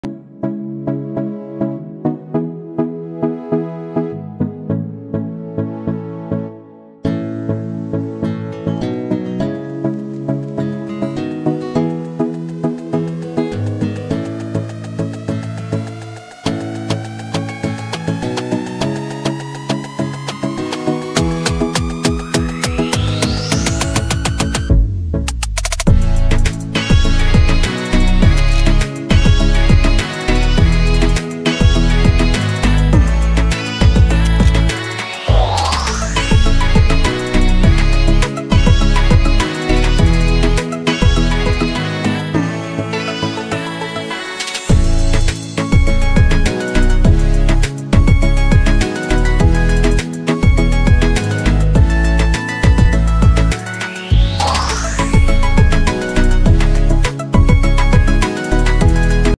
MOBILE PHOTOGRAPHY TIPS | MACRO PHOTO WITHOUT MACRO LENS | PHOTOGRAPHY TIPS AND TRICKS
shot macro picture with mobiles normal camera (5x zoom) without macro lens..